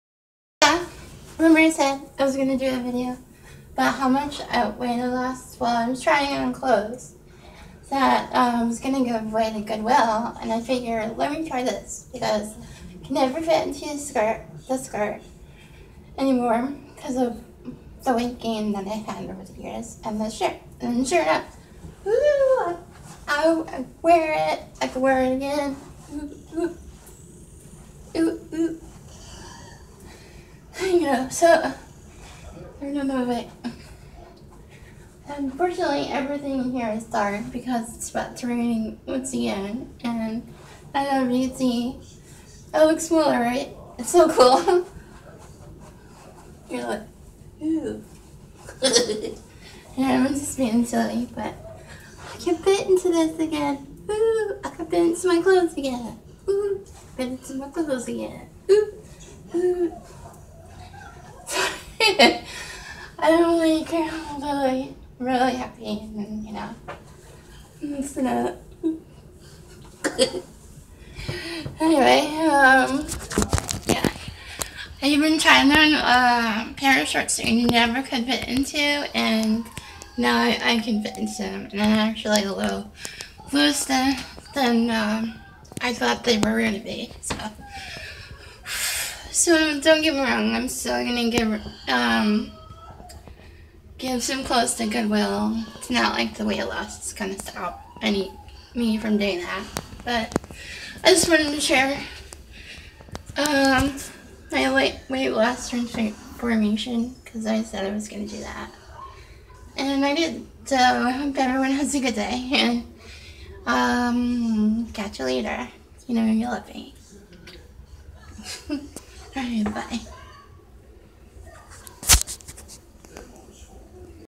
I love you, zombie tits; I lost a lot of weight.
I said I was gonna do a video on how much weight I lost. So here it is.  It's also 5% silly.
Much ❤
An essential tremors warrior. 💪